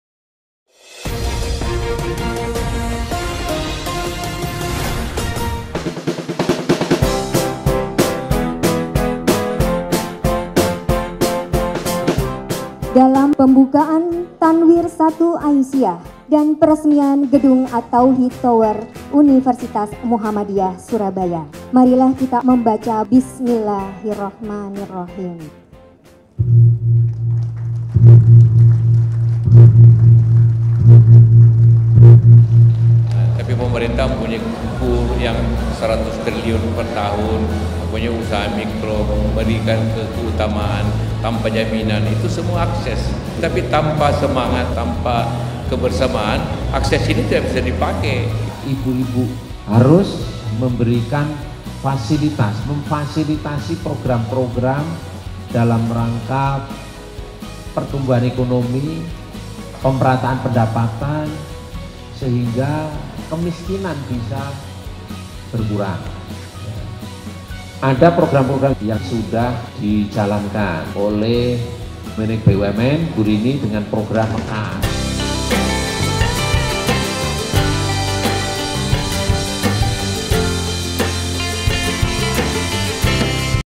Kekuatan Kaum Perempuan Dalam Pembangunan dan Kemajuan Bangsa
Kaum perempuan sosok yang gigih dalam bekerja, pandai memenuhi semua kebutuhan rumah tangga, serta piawai dalam mengatur perekonomian keluarga. Tak mengherankan jika, perempuan menjadi kekuatan potensial dalam pembangunan dan kemajuan bangsa. Termasuk untuk menjadi pelaku gerakan pemberdayaan ekonomi. (Surabaya, 19 Januari 2018)